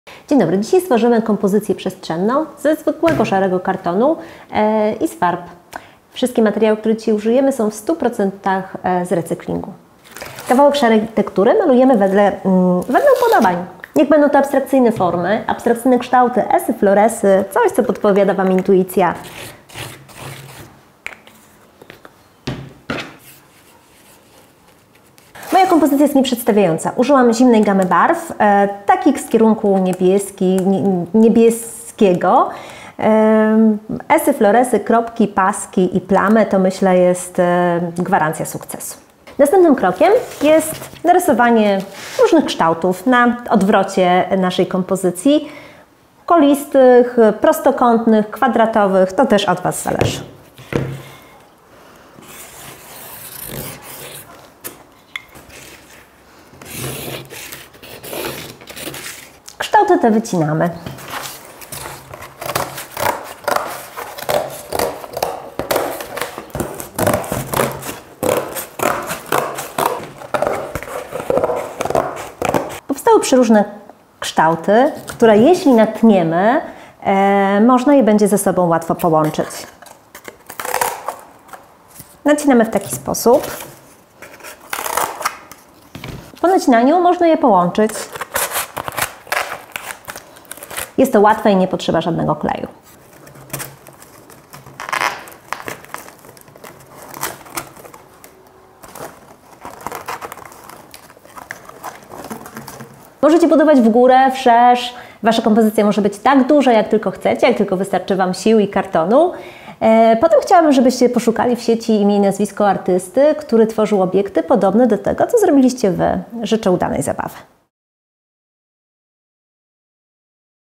Edukacja wizualna - odc. 11
Zrób przestrzenną kompozycję z tektury bez użycia kleju i taśmy. Możesz budować skomplikowane formy, których nie powstydziliby się profesjonalni twórcy!